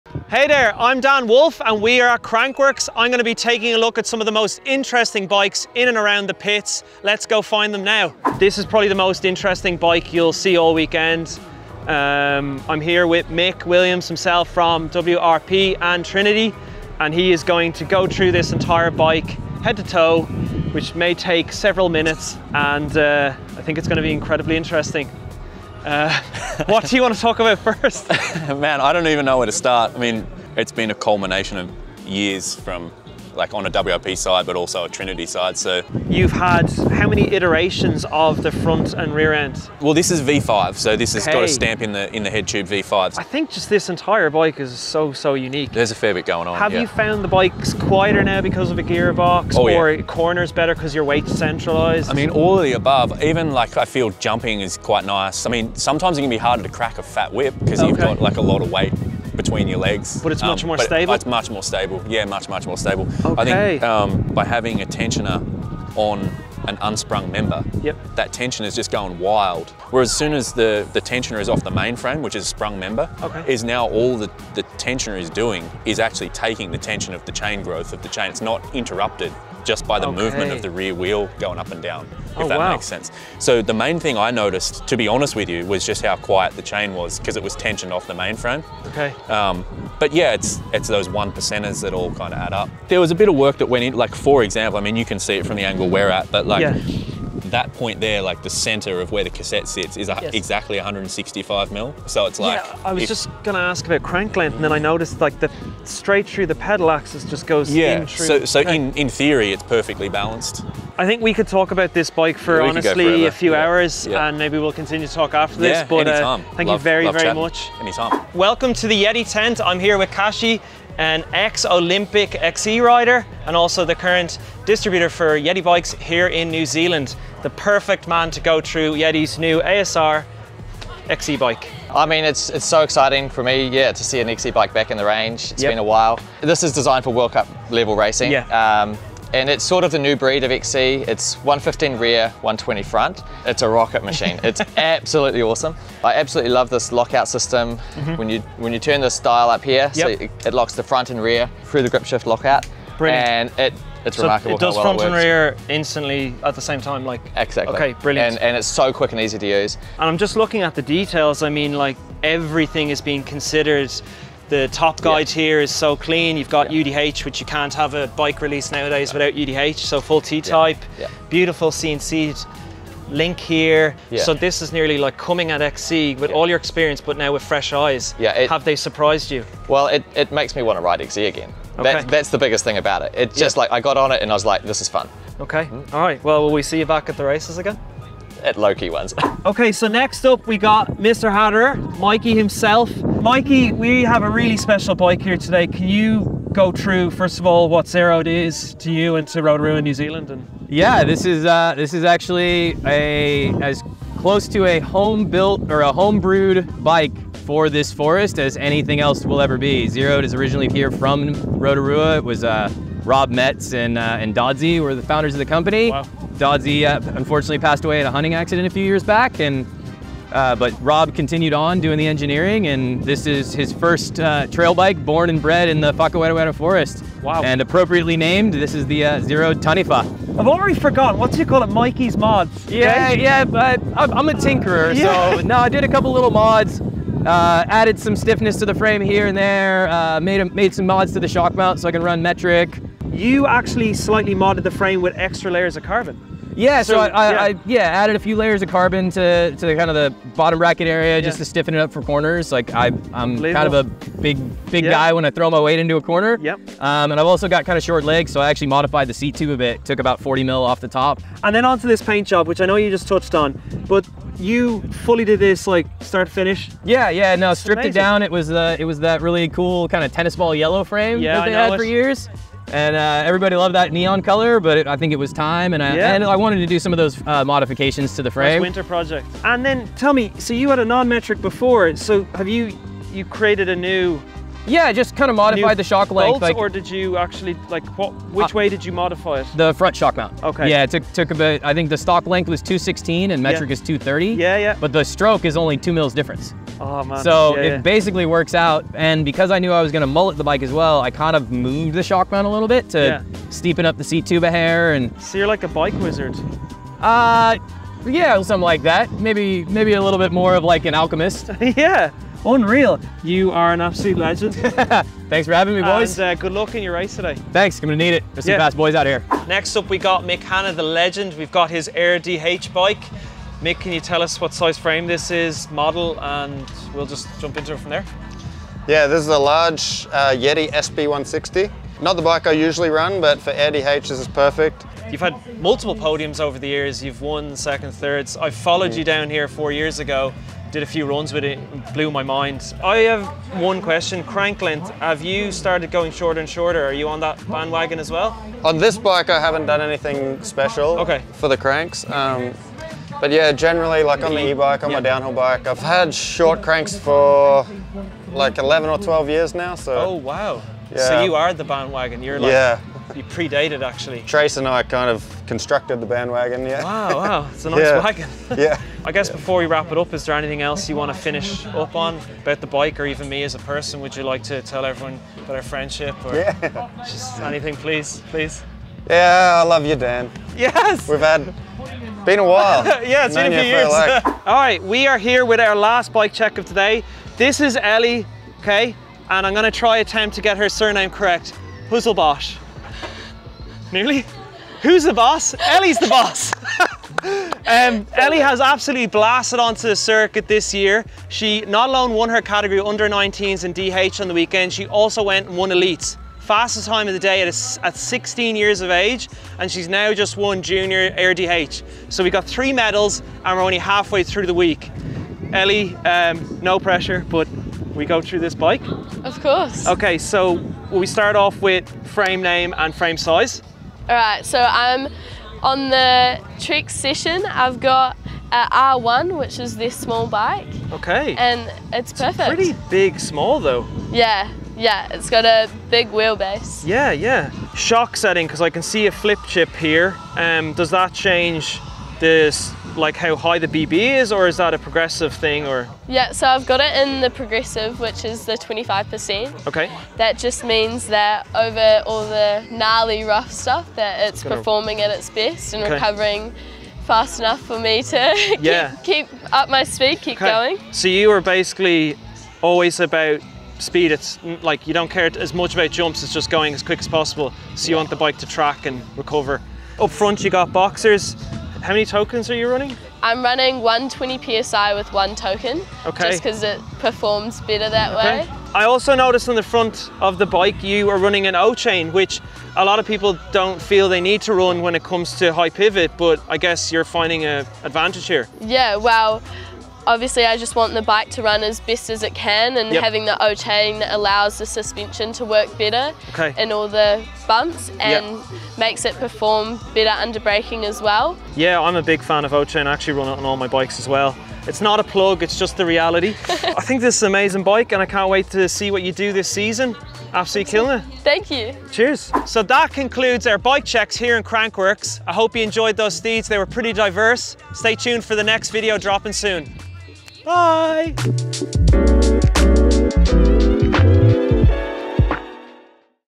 5 Custom Race Bikes Built For Mountain Biking in 2024
There are some great bikes rolling around Crankworx Rotorua right now. Come along with Dan Wolfe and take a deeper look into what mountain bikes your favorite racers are aboard.

00:00 - Intro
00:07 - Trinity Gravity
01:58 - Yeti ASR XC
03:09 -  Zerode Taniwha
05:19 - Yeti SB160
06:23 - Trek Session
08:31 - Outro

Get unlimited access to all the best mountain biking trails by clicking the link above.